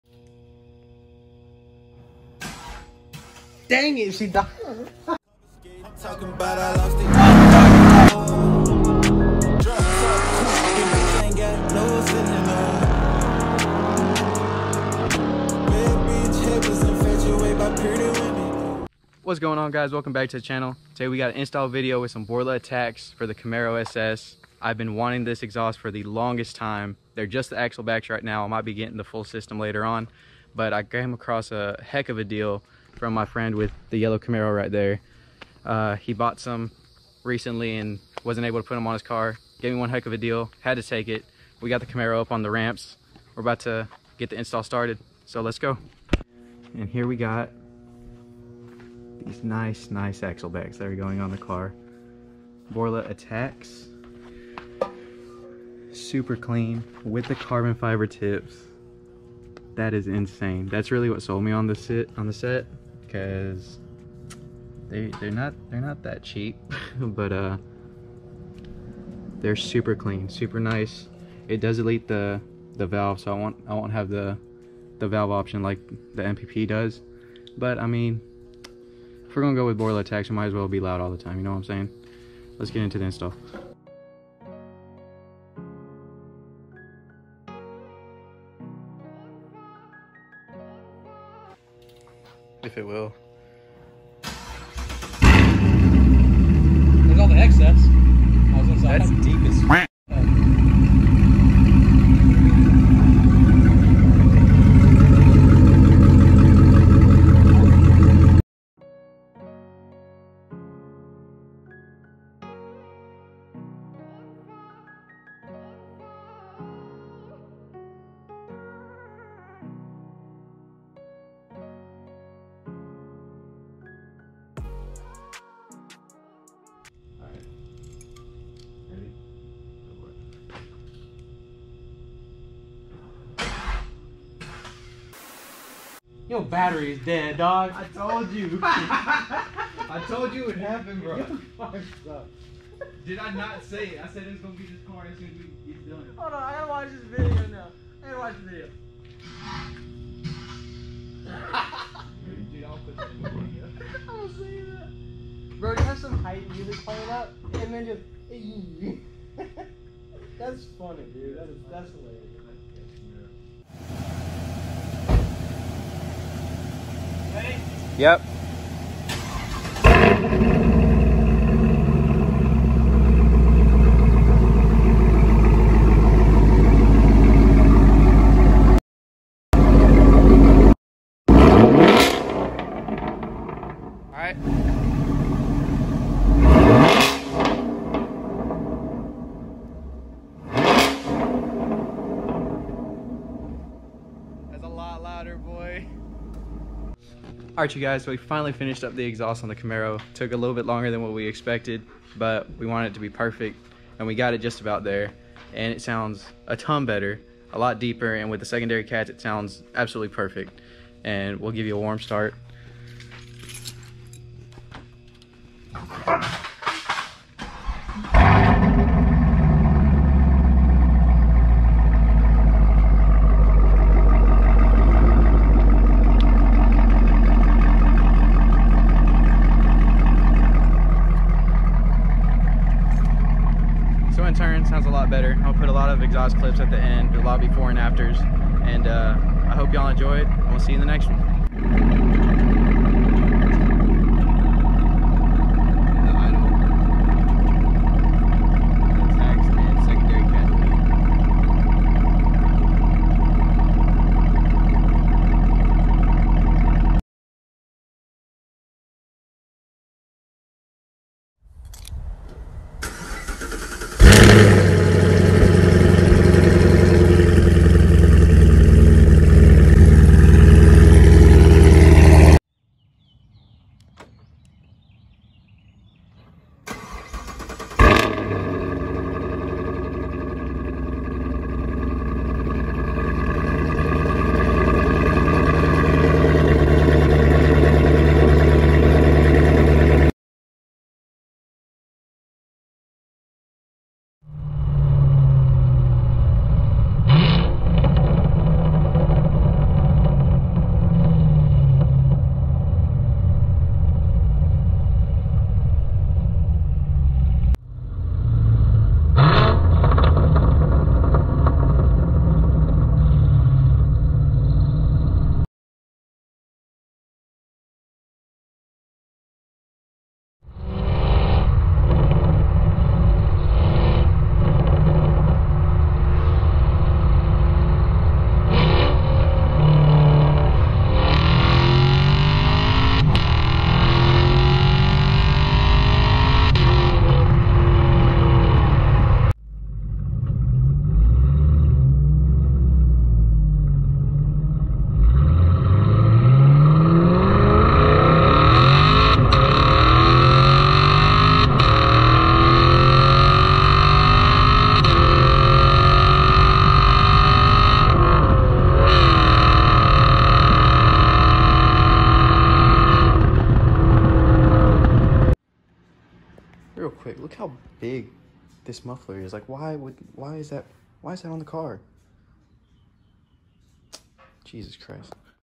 Borla ATAK CF tips 2016 Camaro SS
Finally got exhaust done to the camaro! Borla ATAK with carbon fiber tips. so much deeper and a lot more crackles and pops.

Huge thanks to my boy for hooking me up with a great deal on the axle back